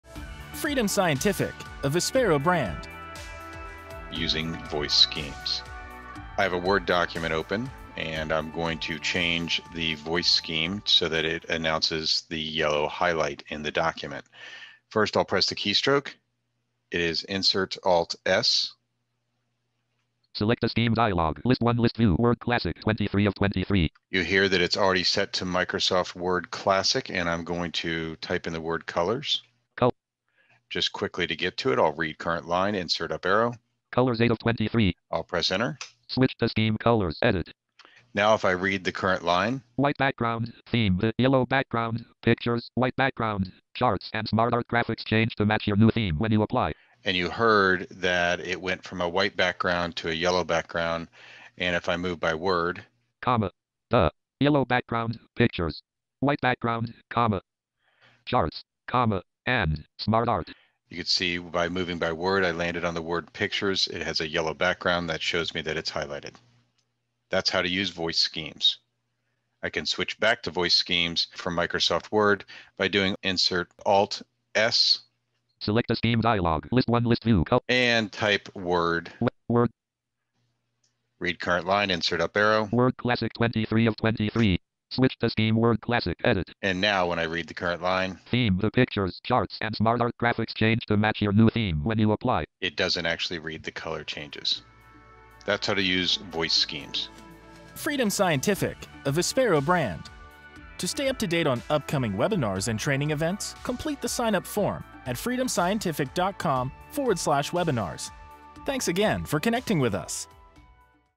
JAWS QuickTips: Using Voice Schemes
Here, Matt Ater demonstrates how to customize JAWS Voice Schemes.